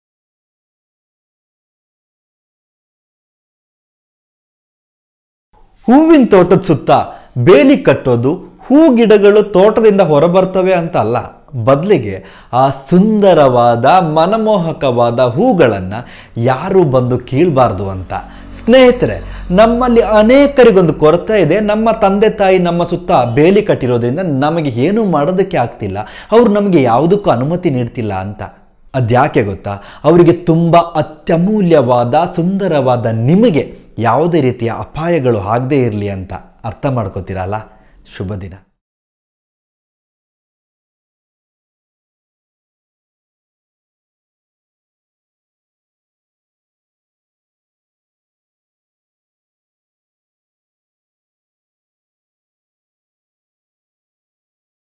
ಅಪ್ಪ, ಅಮ್ಮ ನಮ್ಮ ಸುತ್ತ ಬೇಲಿ ಹಾಕೋದ್ಯಾಕೆ ಗೊತ್ತಾ..? |ಪ್ರತಿಯೊಬ್ಬರು ನೋಡಲೇ ಬೇಕು| sampige vlog
Positive Motivation Tips for Life by Pradeep M P

CHANNEL I FOLLOW
​ Masth Magaa  | Vlog in kannada | Vismaya kannada |Bisccot kannada |sadhguru Kannda | kannada cartoon
@Reference Sreevidhya Santhosh